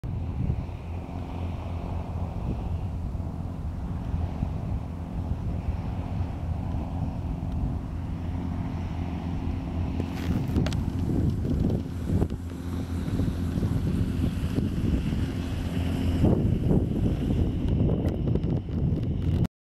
IJseend-vrouw, Stellendam-buitenhaven-KwadeHoek, zo13nov2016
Dobberend en duikend bij afgaand tij.